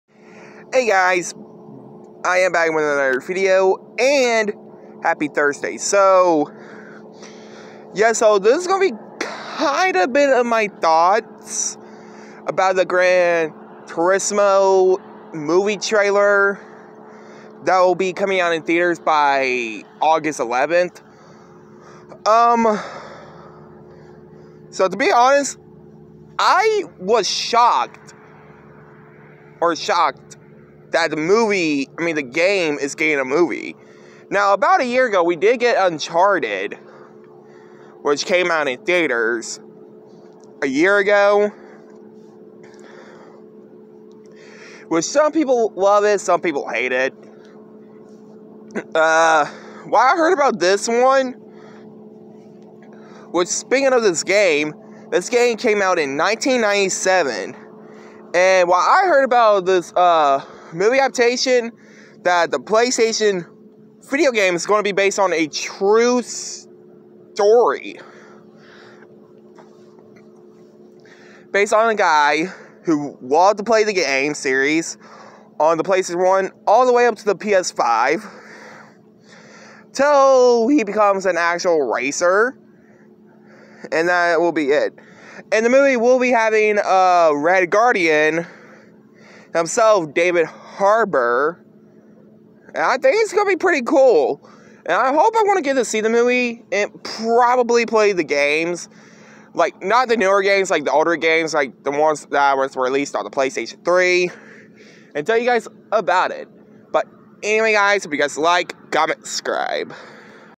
Happy Thursday and My Kind of Thoughts About The Gran Turismo Movie Trailer
What I Heard this Video game live adaptation is going to be based on a true story about this kid who loves playing the Gran Turismo Video Games On The PlayStation And Subscribe To @SuperShockerBrettMesser